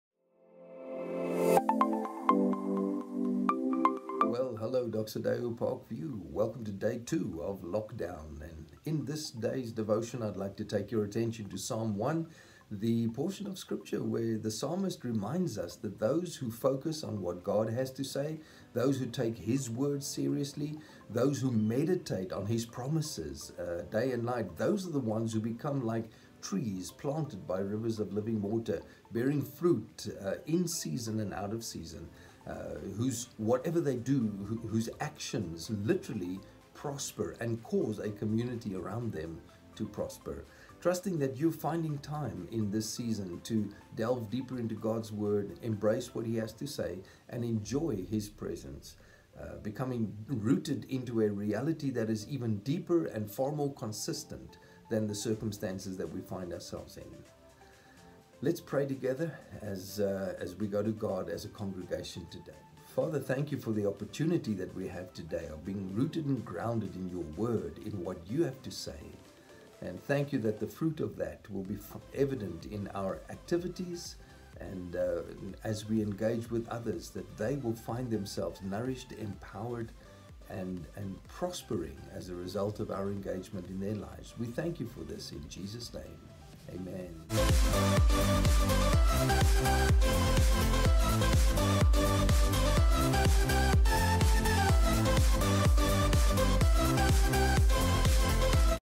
Devotion Day 2 - Rick Moser
As church we want to engage the Word of God during this lockdown season. This is Day 2 as we encouraged from Psalm 1.